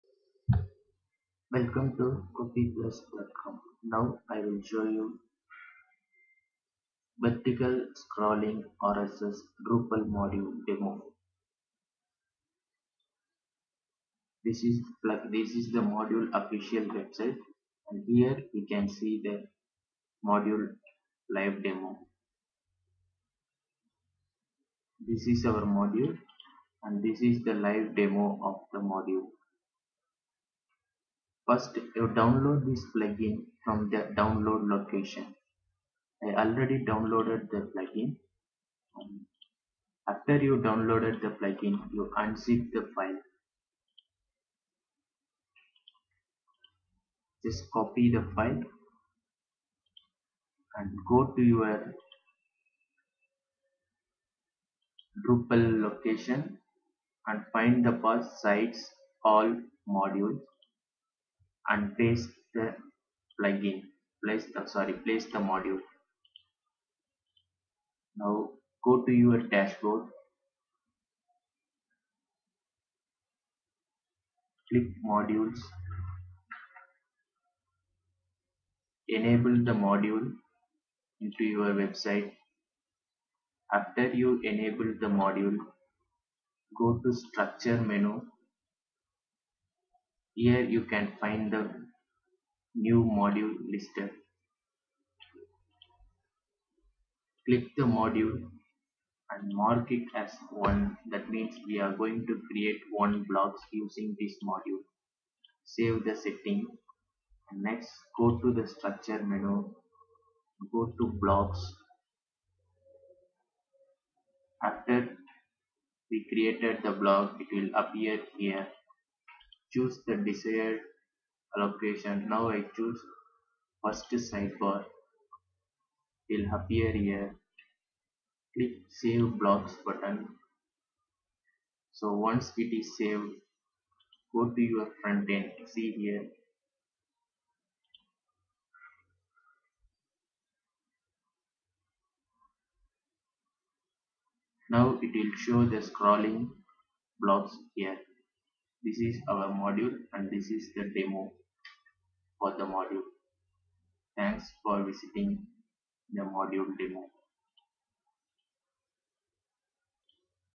Continuous rss scrolling drupal module
Continuous rss scrolling drupal module will create the vertical scrolling reel text show in the drupal website using the given rss feed. This will scroll the rss feed title like reel. The title will scroll one by one in the selected position. Using this module we can show any rss feed in our website. No coding knowledge required to configure this module.